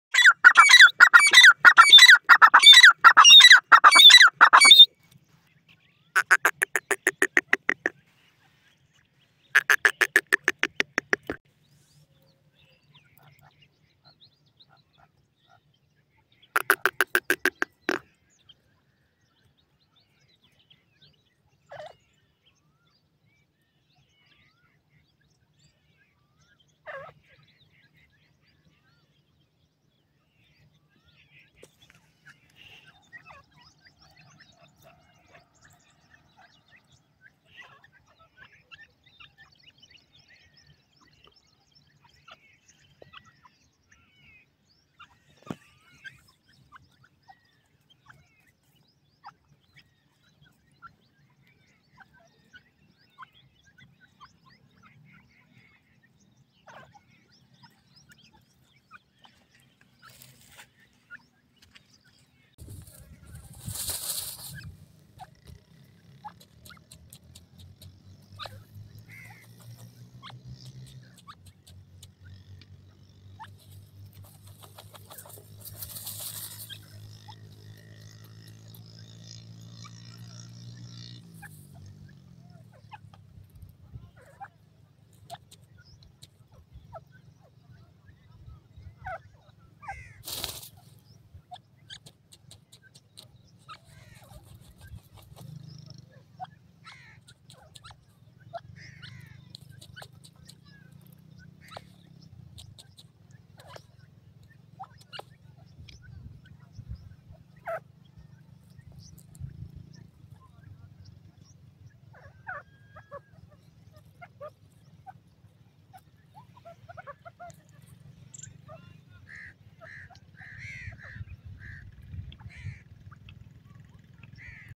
Iranian Teetar in its Natural sound
Set out on a hear-able excursion into the core of Iran's wild, where the charming calls of the Iranian Teetar, or Francolin, anticipate. Let the mitigating songs of this subtle bird transport you to a domain of peacefulness and tranquility.

This video catches the credible hints of the Iranian Teetar right at home, giving a brief look into the captivating universe of this puzzling bird.
The Teetar's unmistakable calls, going from delicate trills to energetic whistles, illustrate its presence in the Iranian scene.

Whether you're a nature fan, a bird darling, or basically looking for a snapshot of harmony and unwinding, this video will without a doubt enrapture your faculties.
Submerge yourself in the captivating hints of the Iranian Teetar and rediscover the agreement of nature.